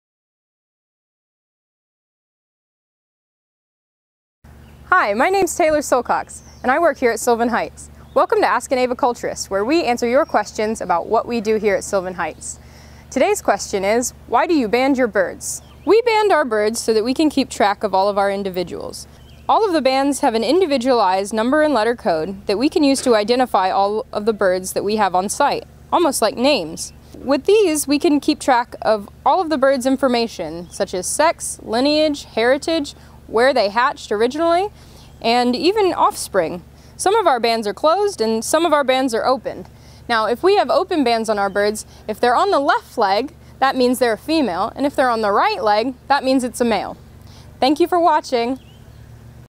Ask An Aviculturist - ID Bands
Why do the birds at Sylvan Heights wear bands on their legs?